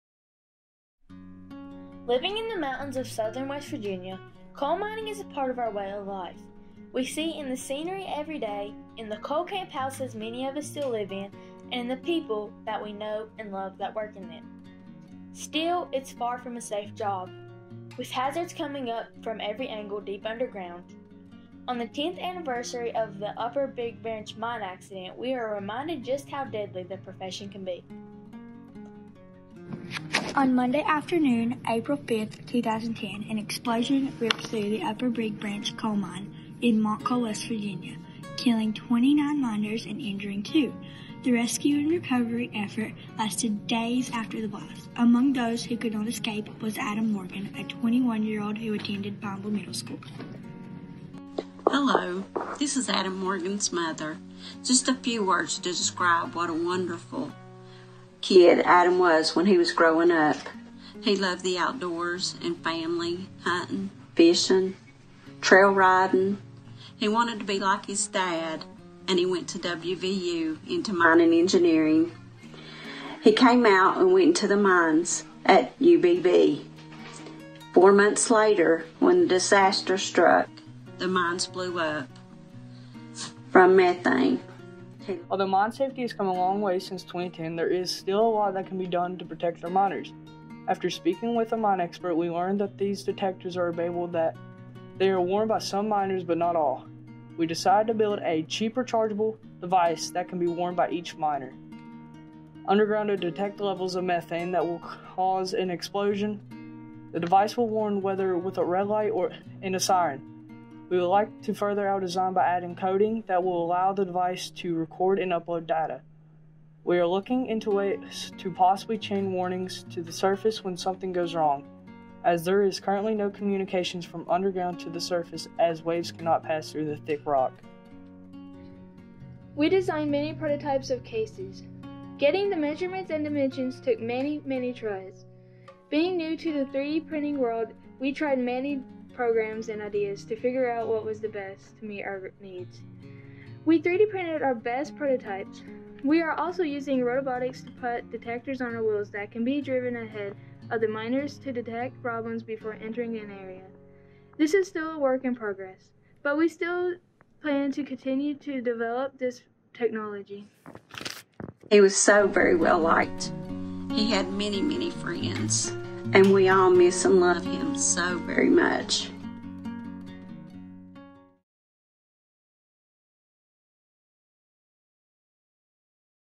2020-2021 Solve for Tomorrow Student Video: Pineville Middle School, West Virginia
As part of their Samsung Solve for Tomorrow project, students at Pineville Middle School in Pineville, WV used STEM to design a device that detects high levels of methane underground to protect local miners working in hazardous condition.